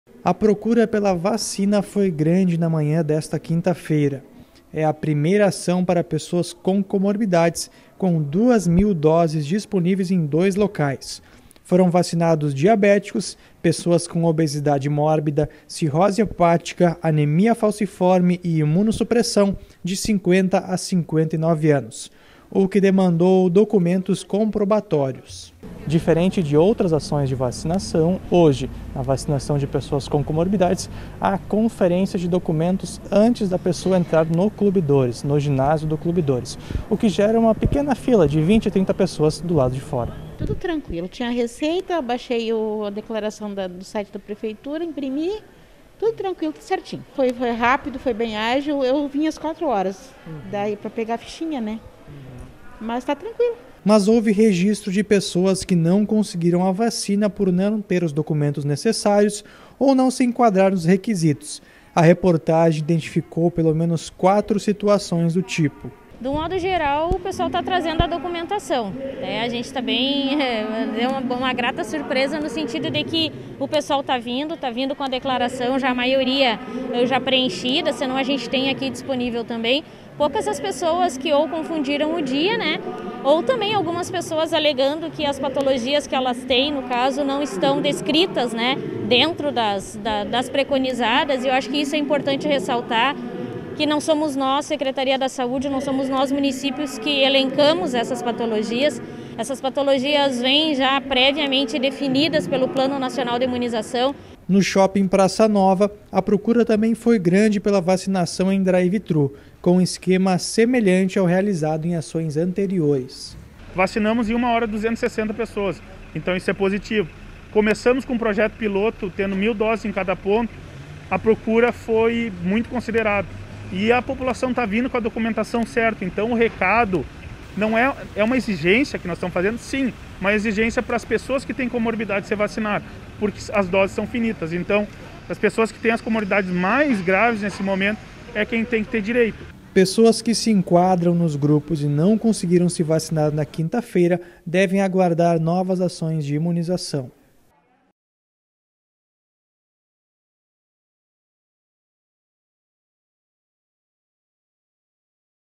Vacinação para pessoas com comorbidades começou nesta quinta em Santa Maria
2 mil doses foram disponibilizadas em dois pontos da cidade